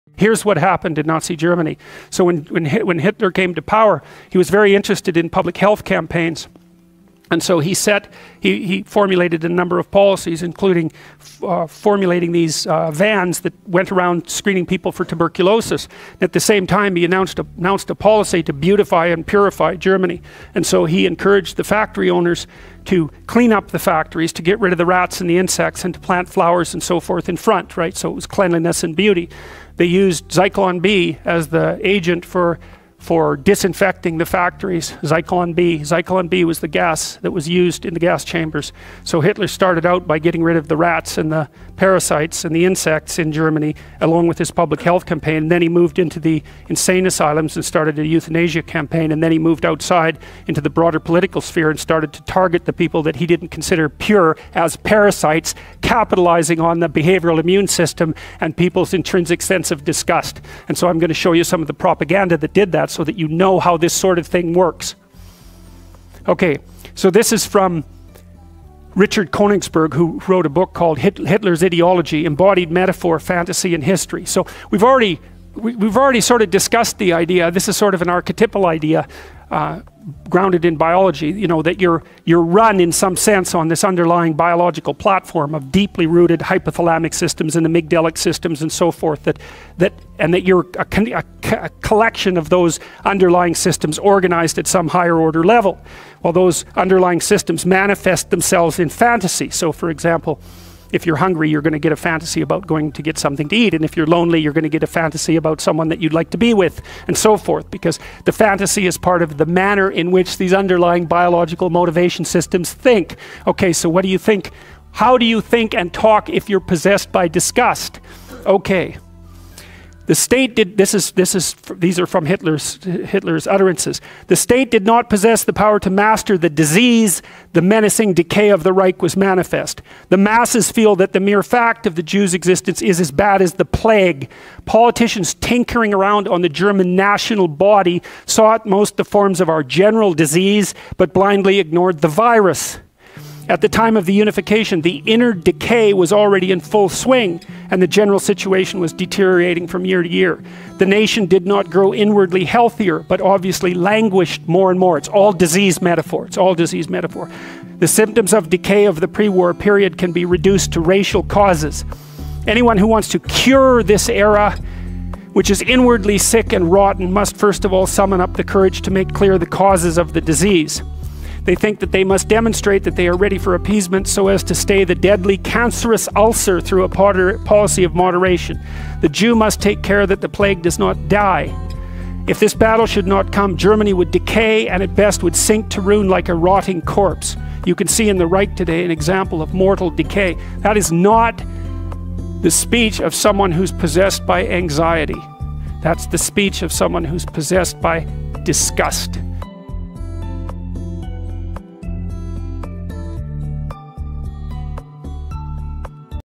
jordan peterson : hitler moved from exterminating pests to exterminating people
Musicien: @iksonmusic
you can watch all video from jordanbpeterson channel on youtube